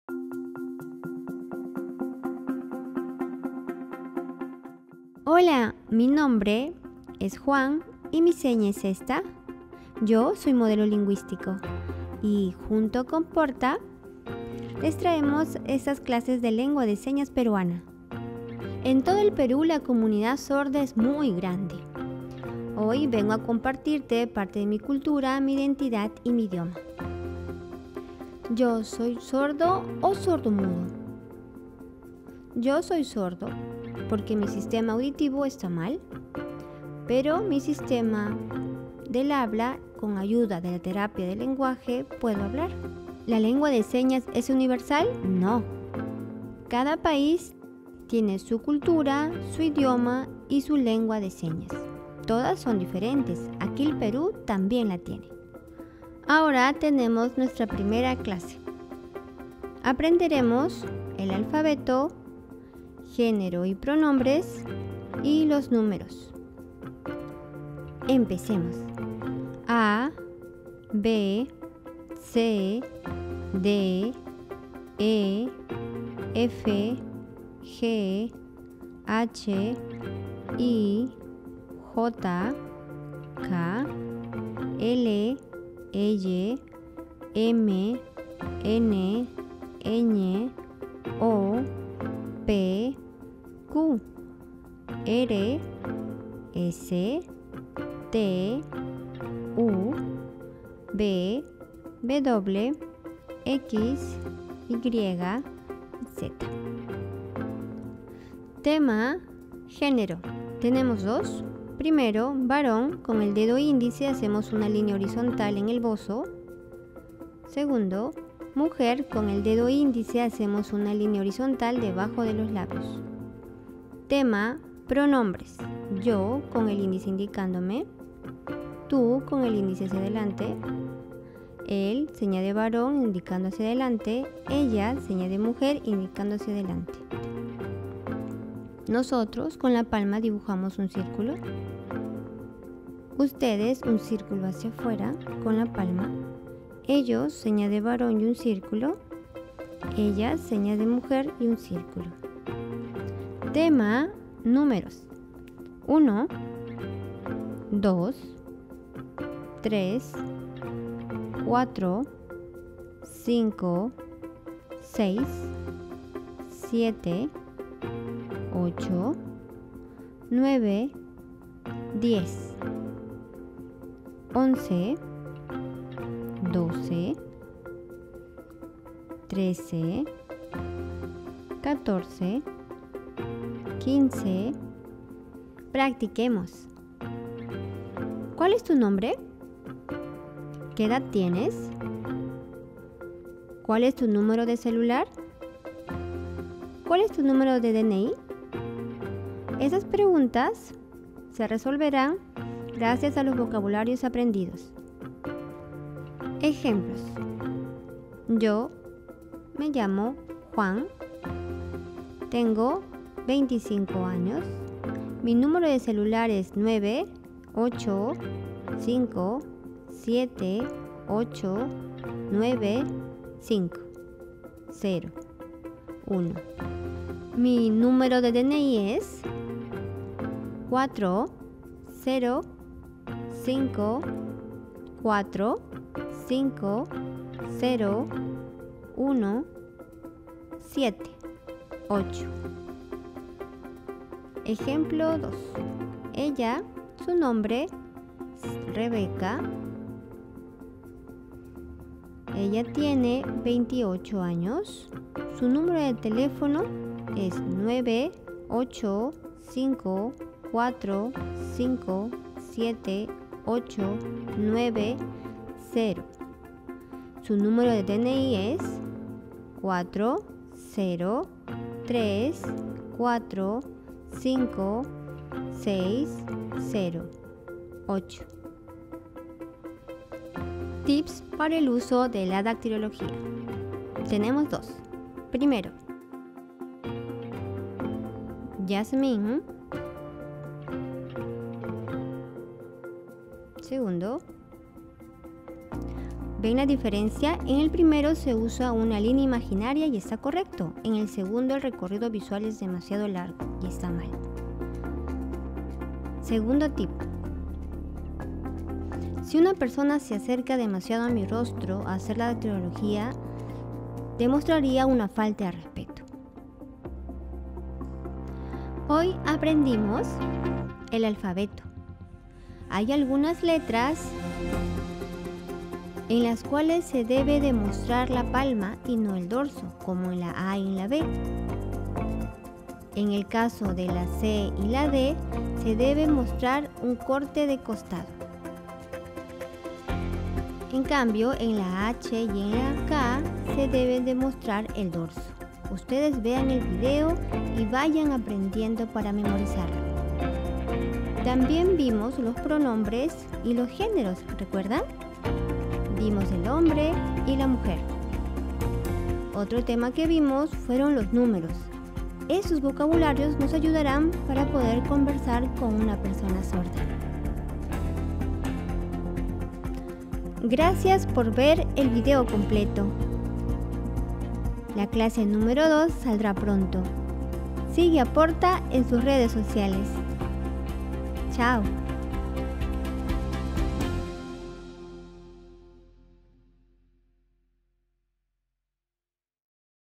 "APRENDO CON PORTA" - "LENGUAJE DE SEÑAS" SESIÓN 1
¡Aprende "Lenguaje de Señas" junto a nosotros desde cero!
Estamos comprometidos con la inclusión social y la importancia de conocer el lenguaje de señas para ser un país más inclusivo en donde sea más fácil comunicarnos y conocernos entre todos. ✨🫂👋